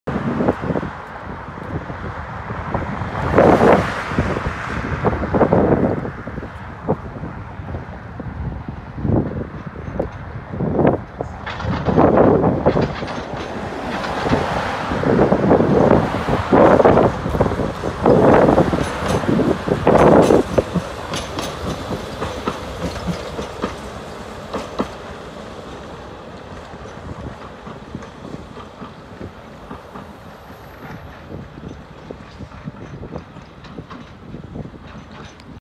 71-414 3502 На Строгинском мосту
Трамвай был построен 02.2014

ОПИСАНИЕ

Поступил Краснопресненское трамвайное депо

История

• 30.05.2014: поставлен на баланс